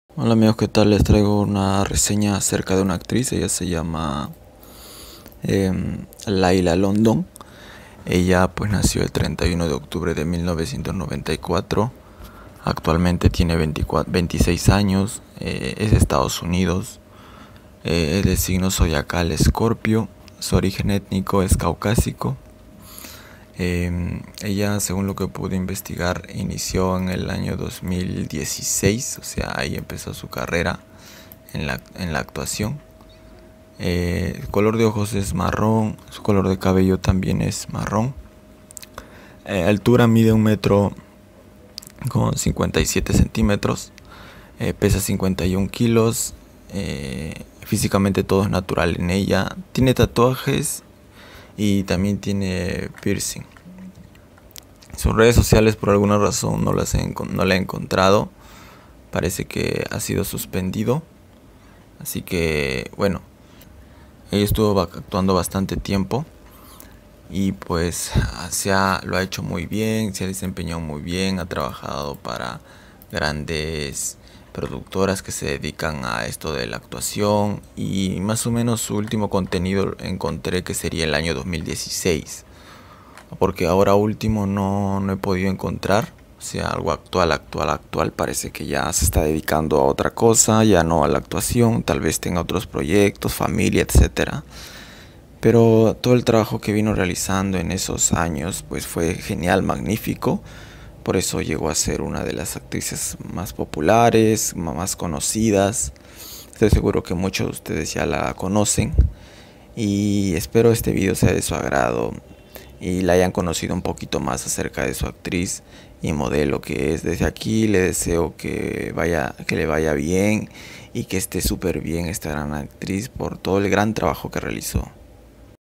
LAYLA LONDON | THE 2016 ACTRESS WHO WORKED EXCELLENTLY TO BE SUCCESSFUL
In this video I will review a model who played many characters and managed to succeed.